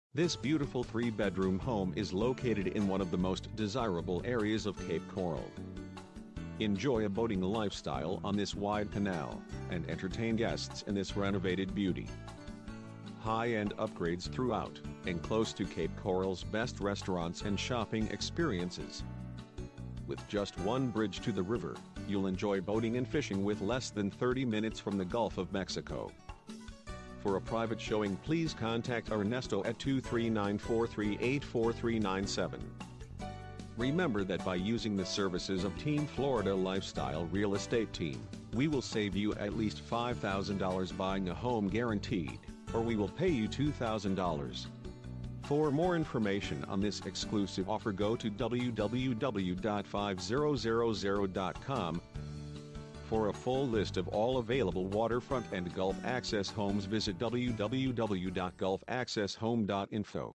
Beautiful Gulf Access Home - 621 SE 28th Ter Cape Coral FL 33904
Gulf access - Just one bridge to the Gulf. Beautiful single family rooms on desirable location in SW Florida